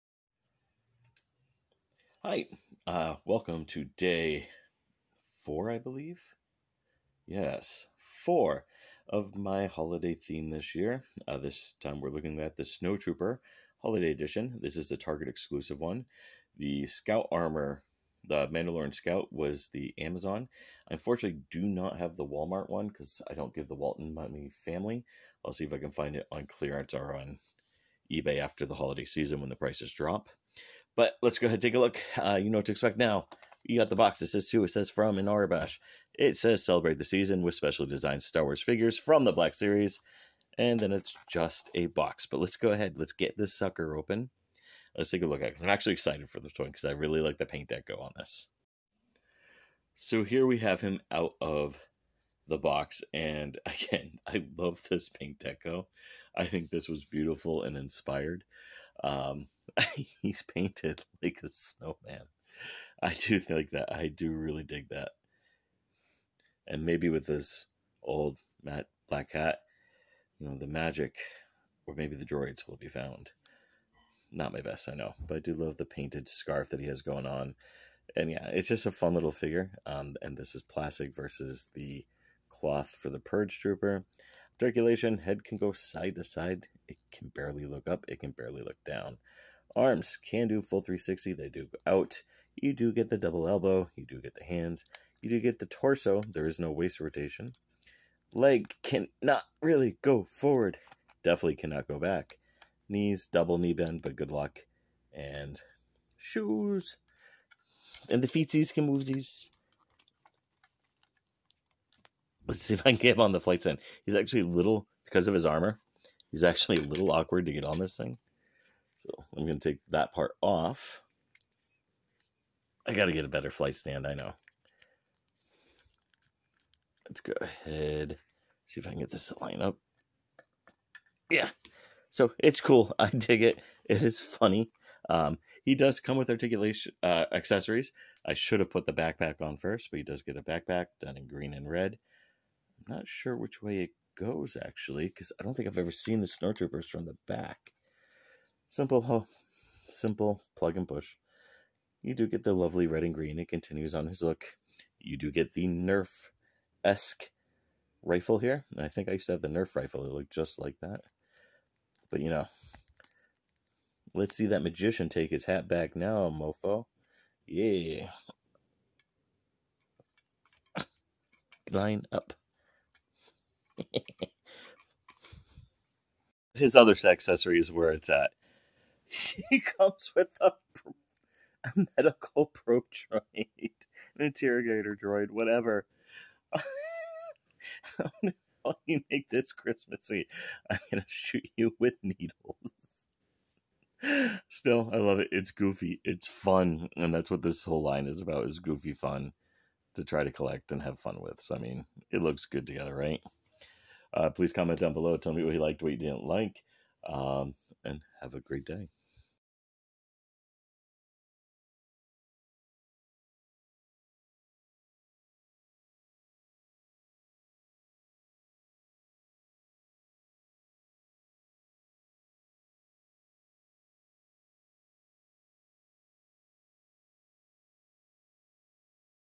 Star Wars The Black Series Holiday Edition Snow Trooper and Interrogation Droid
Welcome to day 4 of the Holiday Theme.  Today is the Snow Trooper review.  About the thumb nail, I am using the interrogation droid to build his trooper snowman.